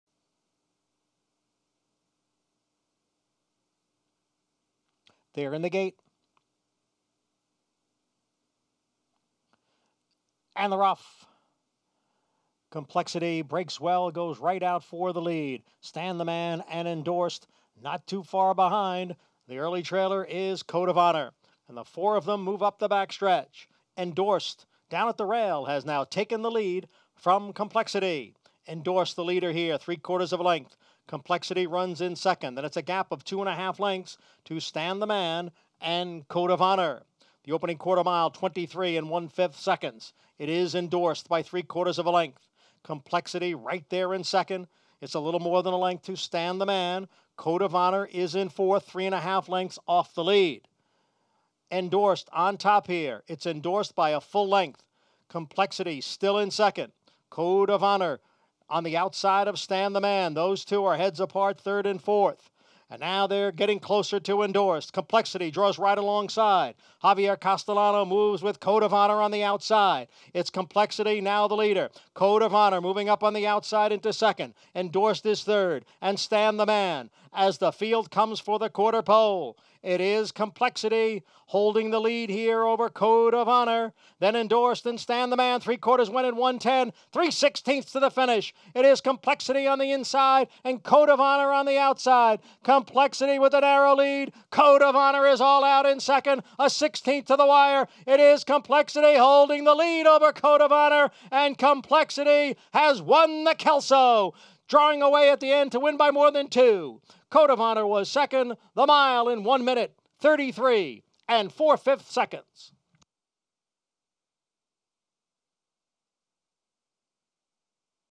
Complexity - 2020 - The Kelso
Klaravich Stables' Complexity kept an unbeaten record going a one-turn mile at BelmontPark intact when tracking the pace and shaking off a late challenge from Code of Honor to capture the 40th running of Saturday's Grade 2, $150,000 Kelso for 3-year-olds and up on the main track.